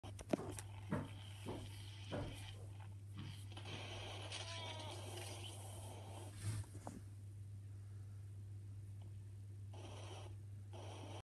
What happens when you play as Irresponsible Dad on the Jet Fall level
I was bored lmao